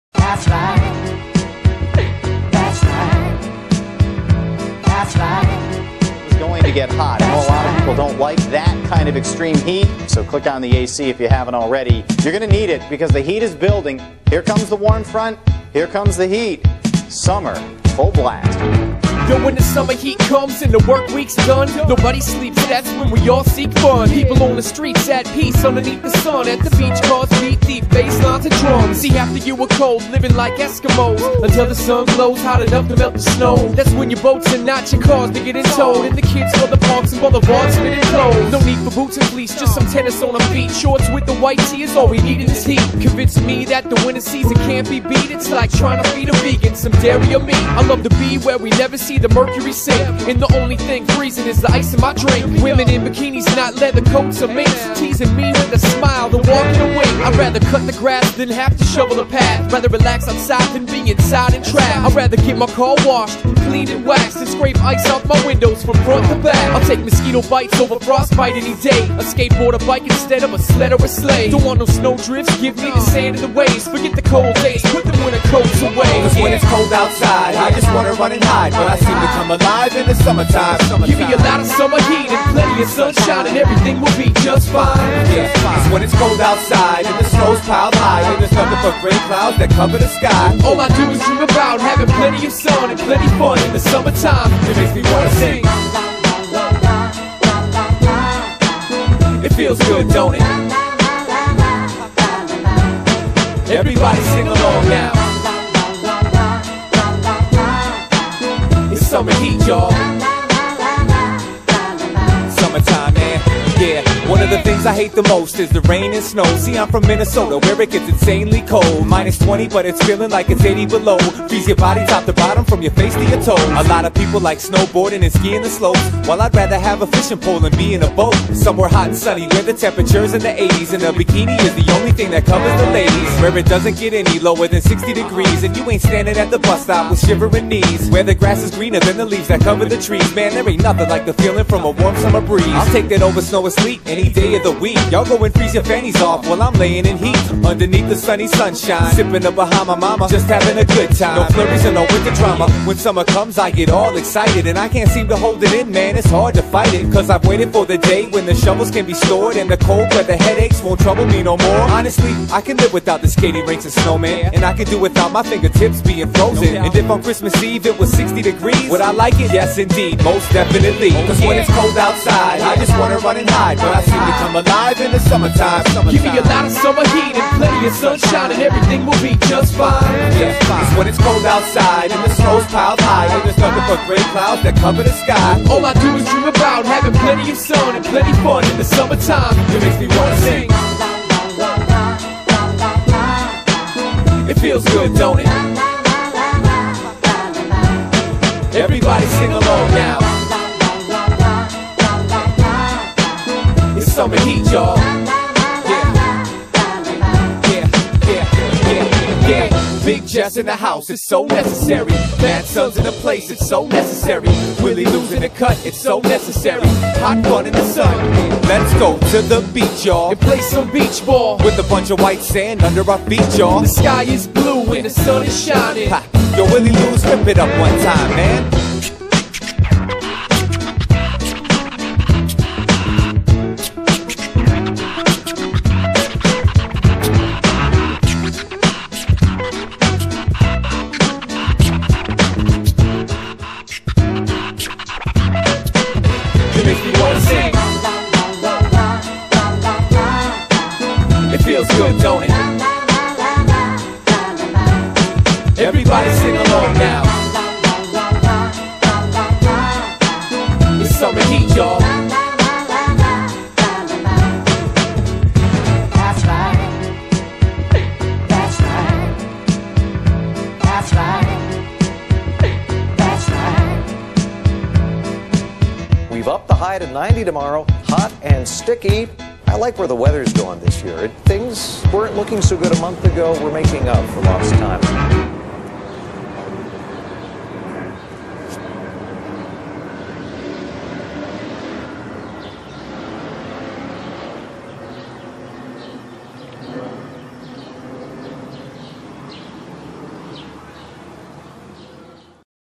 Minneapolis/ Downtown, Dinky Town, North side 6-25-12
Did some Skateboarding and took some Photos Near Downtown Mpls.